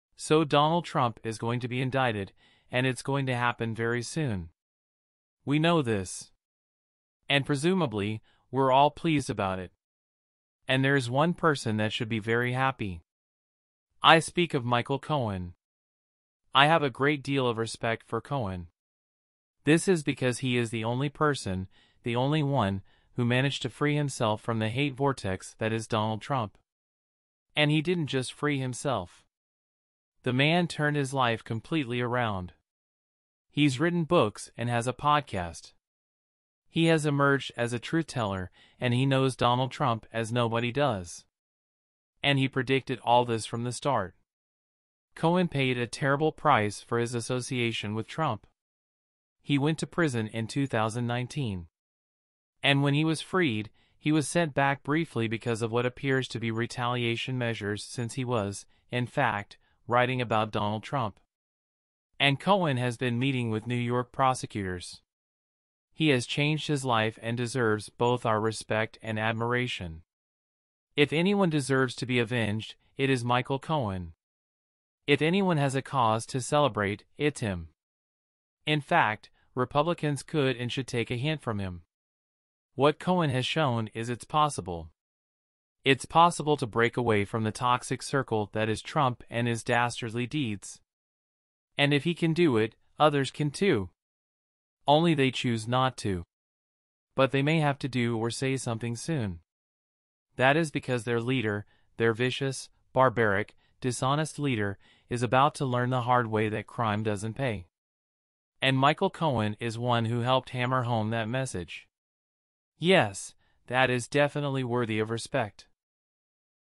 Michael Cohen’s day in the sun
So Donald Trump is going to be indicted, and it’s going to happen very soon. We know this. And presumably, we’re all pleased about it. And there is one person that should be VERY happy. I speak of Michael Cohen.